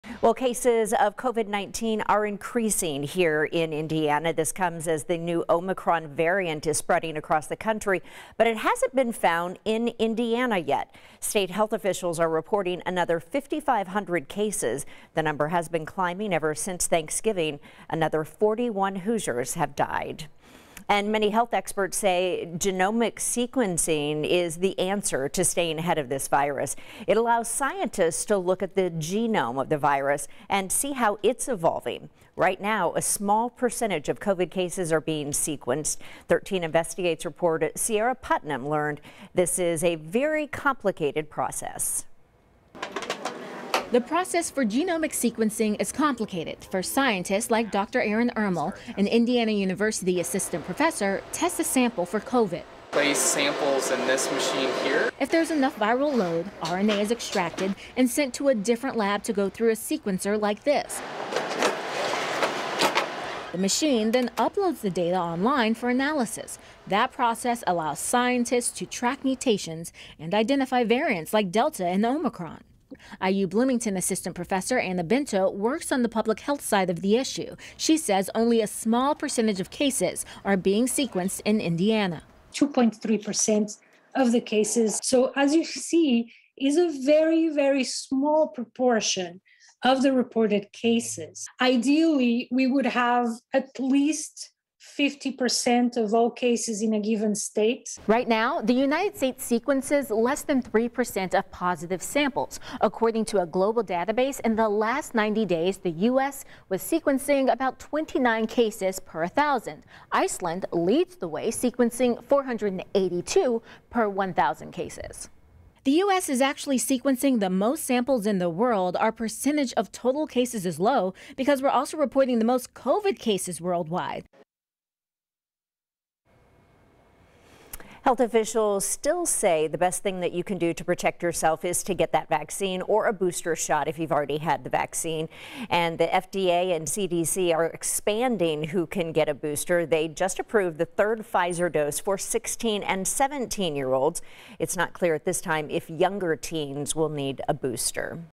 Indiana coronavirus updates: 12/10/2021 Sunrise update
Indiana coronavirus updates: Indiana numbers, explaining genome sequencing of COVID-19, Pfizer booster dose approved for 16-year-olds — 12/10/2021 Sunrise update